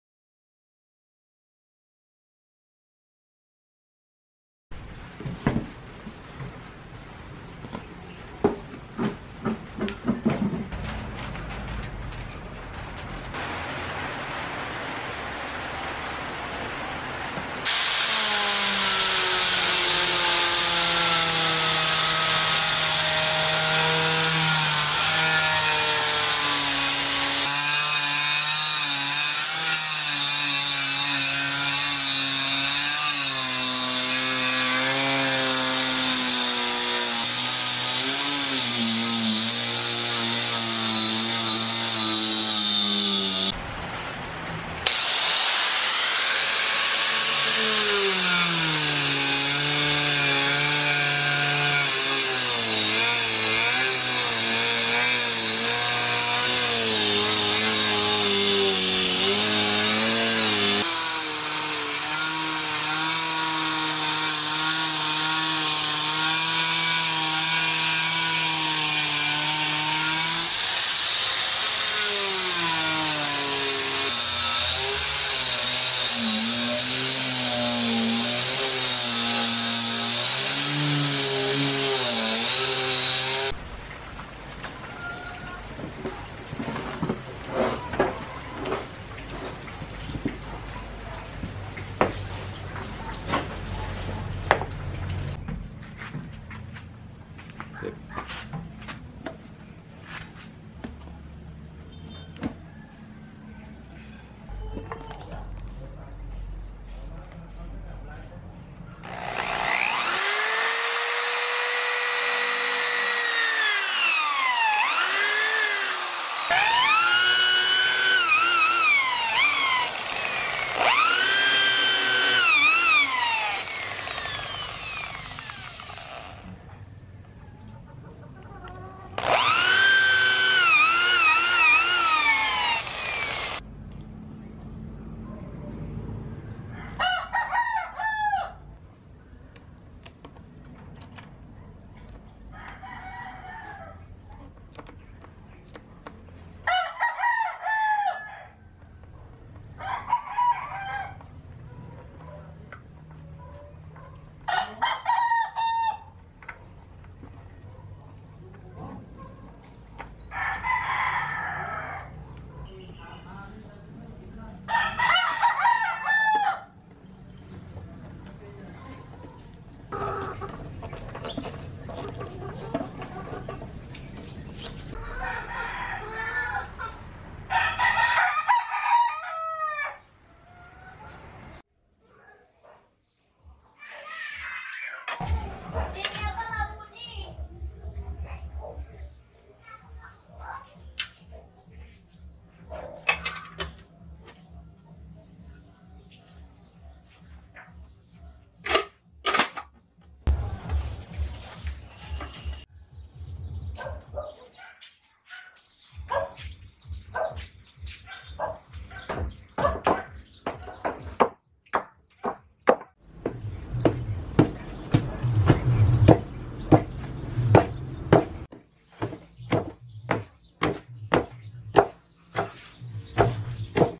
How to Make Homemade Punching Bag From Old Tire
Homemade Tire Punching Bag - Awesome Gym Ideas
Cheap and easy homemade punching bag.